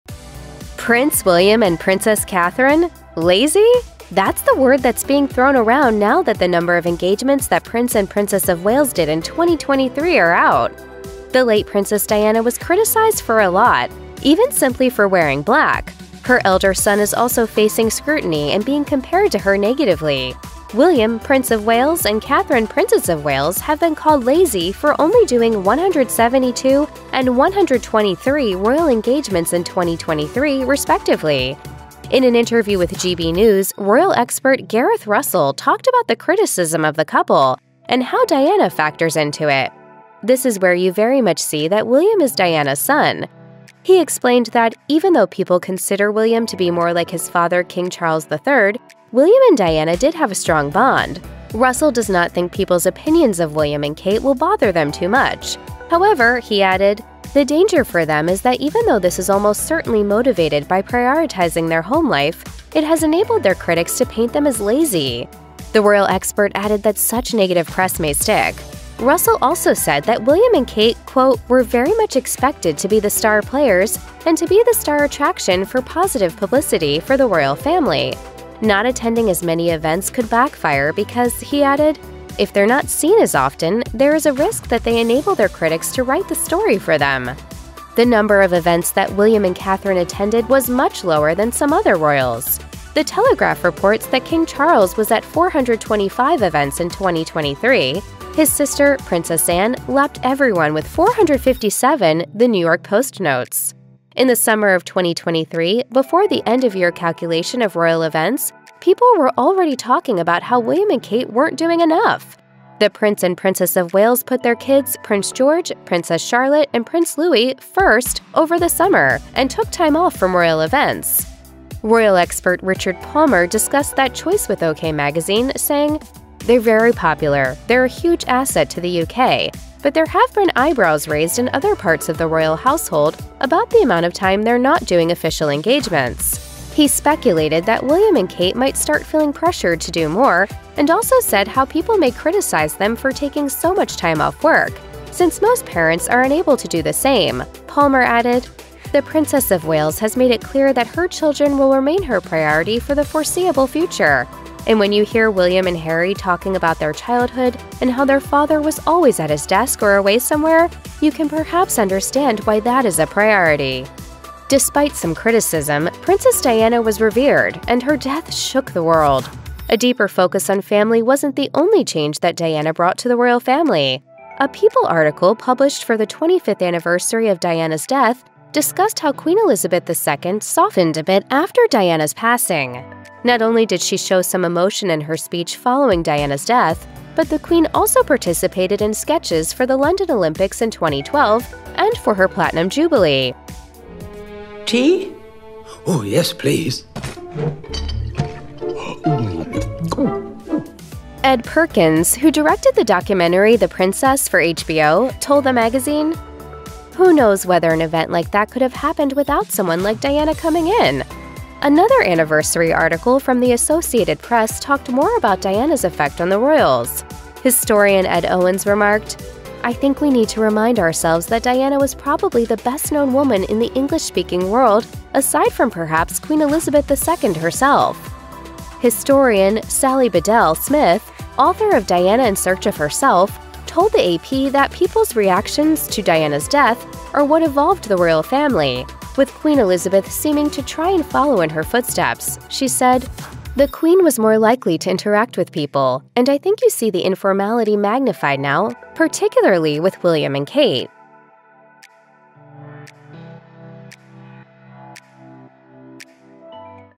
Buckingham Palace Isn't Too Happy With Kate & William
Prince William and Princess Catherine…lazy? That's the word that's being thrown around now that the number of engagements that Prince and Princess of Wales did in 2023 are out.

Voiceover By: Chrissy Baker